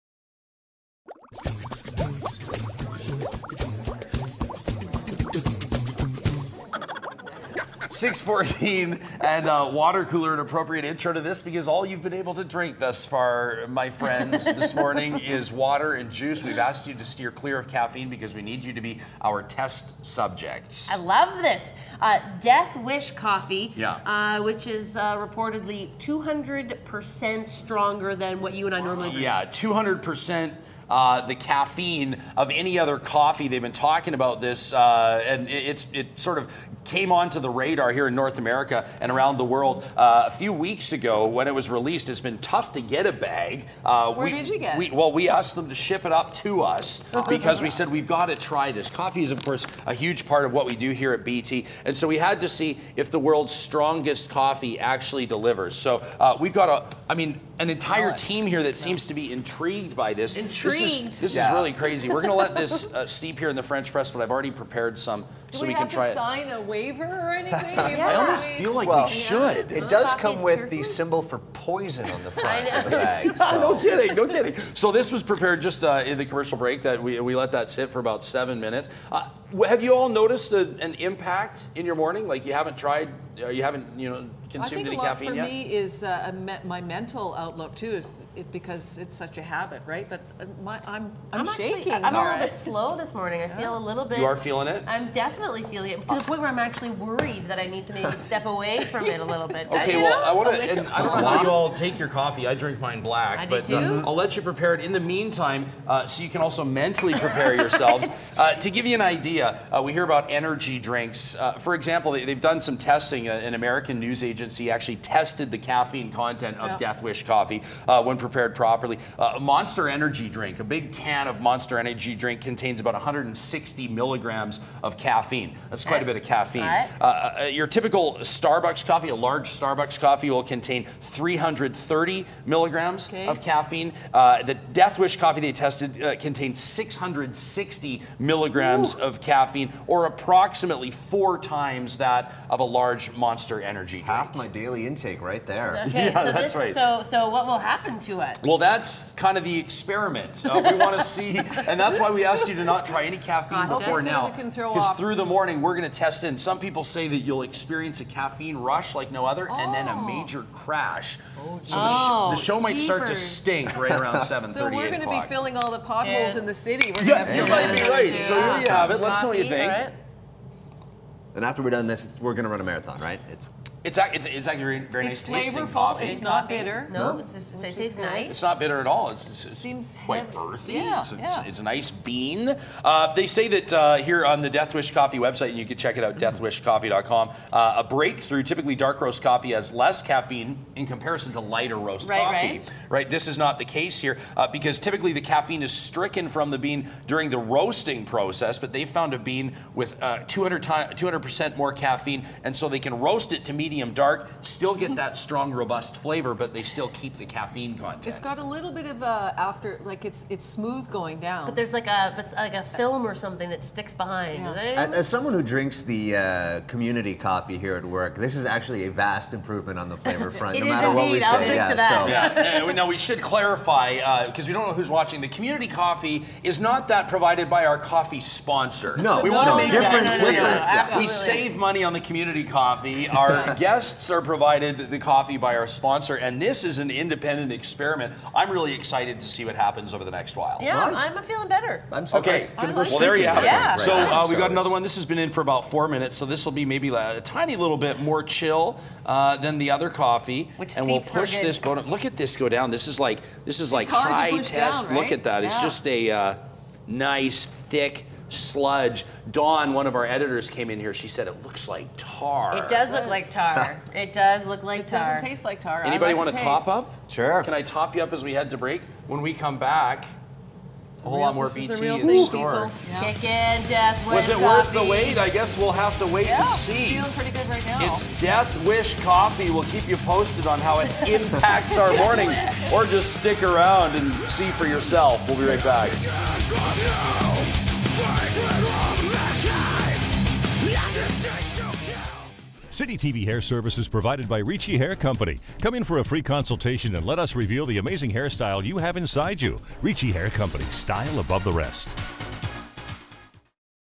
Death Wish Coffee
Our BT crew tries some Death Wish Brew.